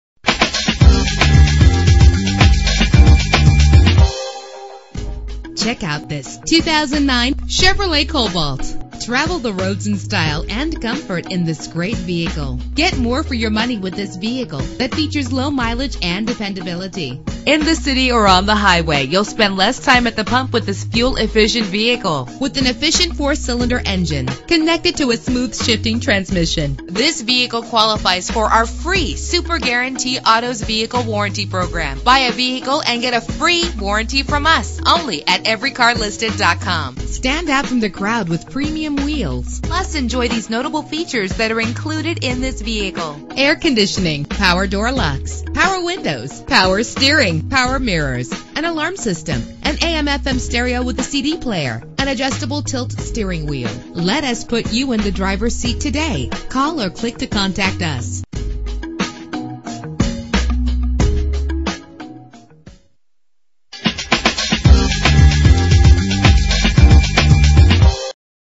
2009 Chevrolet Cobalt - FONTANA CA
Year: 2009
Make: Chevrolet
Model: Cobalt
Trim: LT
Engine: 2.2 liter inline 4 cylinder 16 valve
Transmission:
Color: Red
Mileage: 14152
Address:
16005 VALLEY BLVD